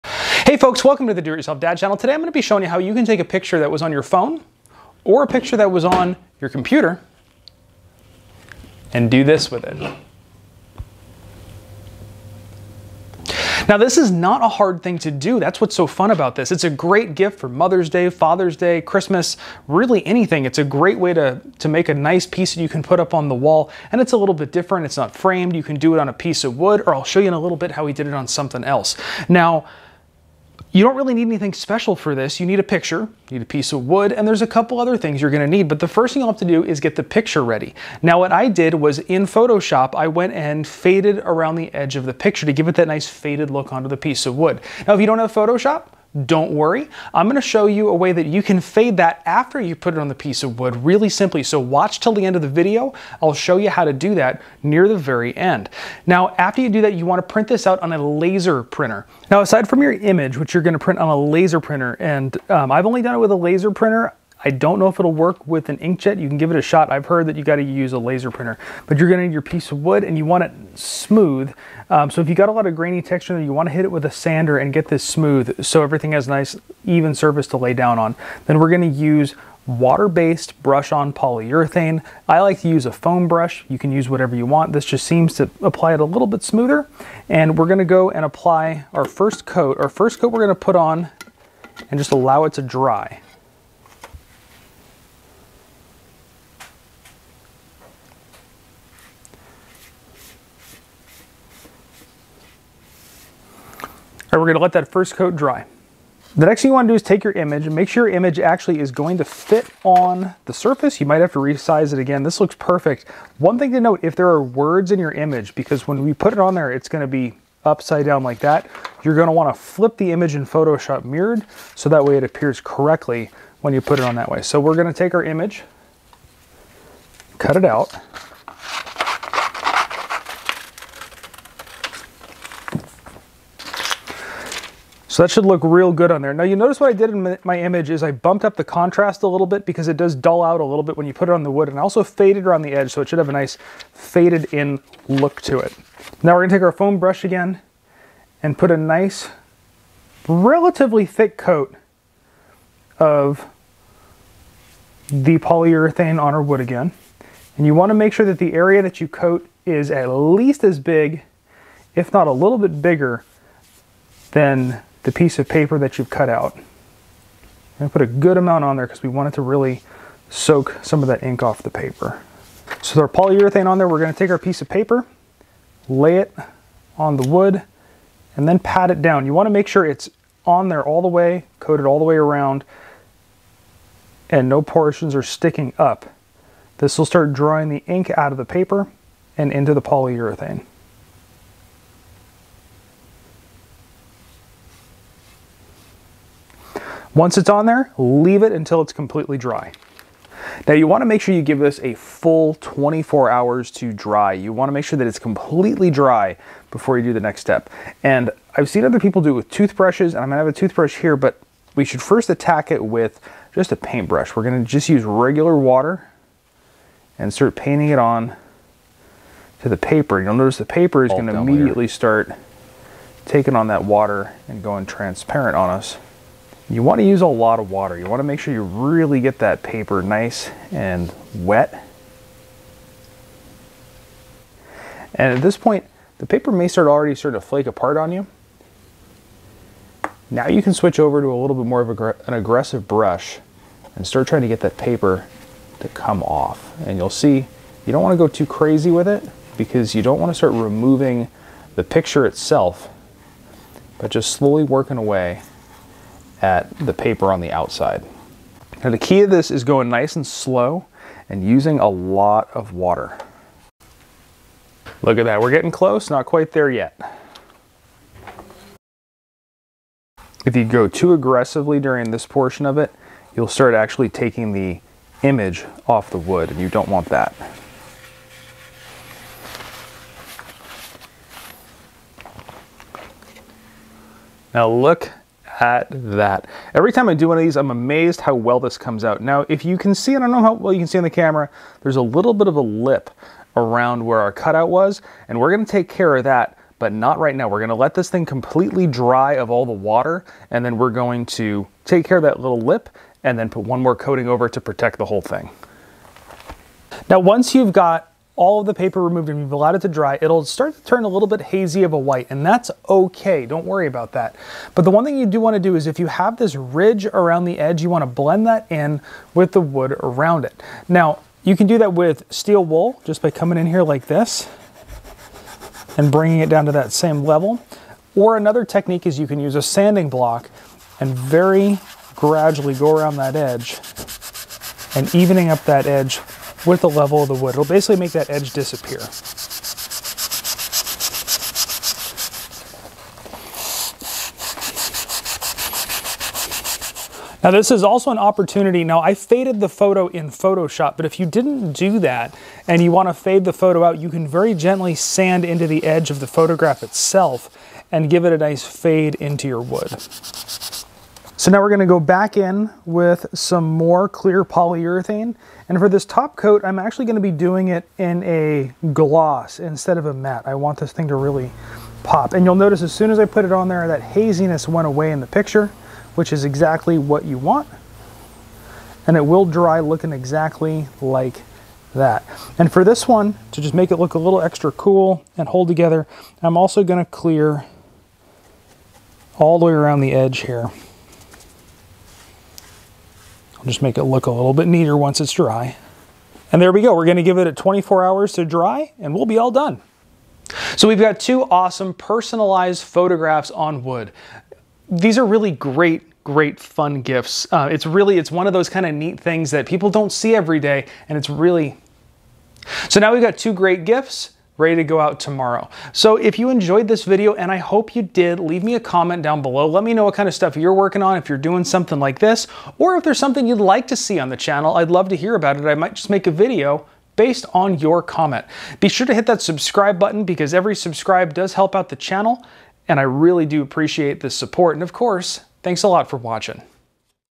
Easiest Way To Transfer A Photo To Wood
The easiest way to transfer a photograph to wood. Easy and safe! All you will need is:
A photo printed with a laser printer
A piece of wood
Water-based polyurethane
Paint Brush
Sanding Blocks

Get this, and all the things we've used on DoItYourselfDad